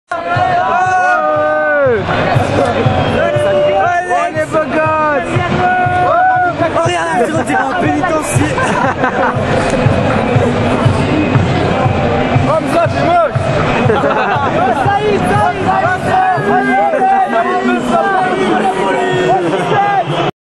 ramasseurs
rentré des ramasseurs de la ssa dans le stade